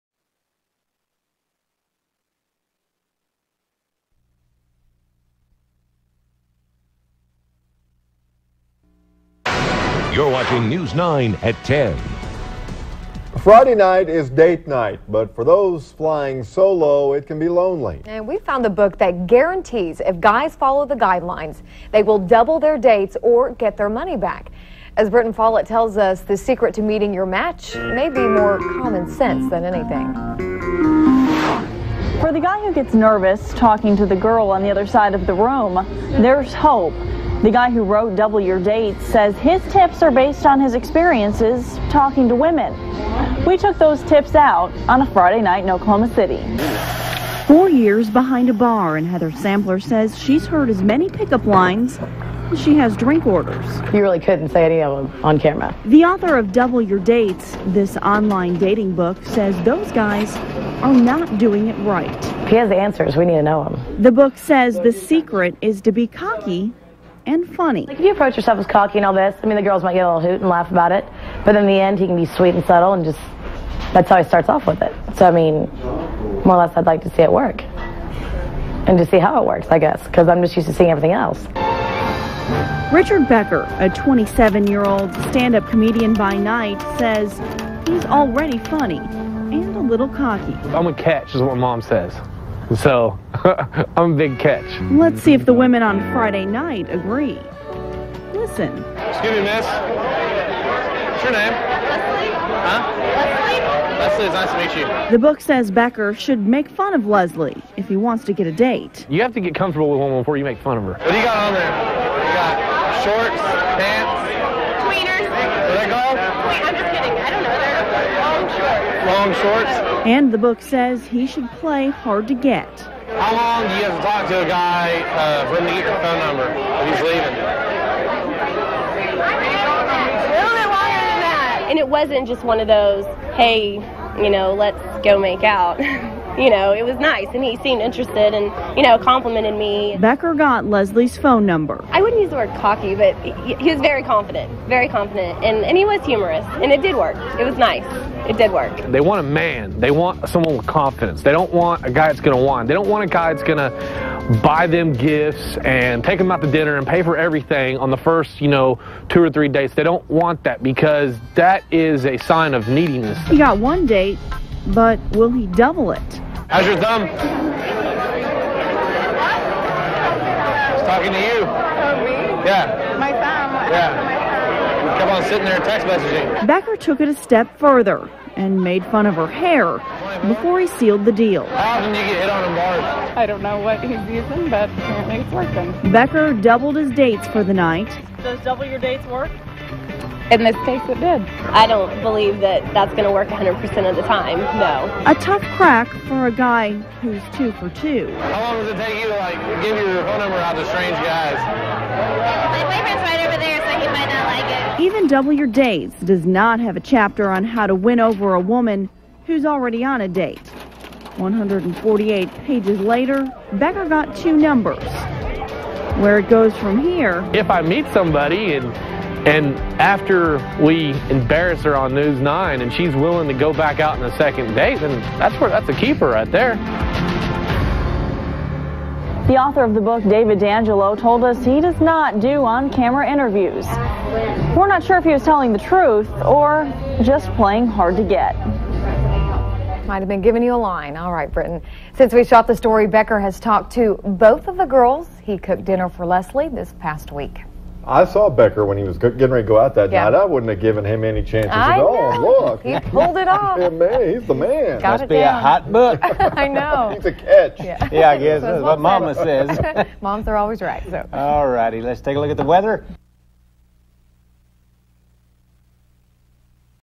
Double Your Dates: Richard Becker
Richard out in the field showing how to pick up the LADIES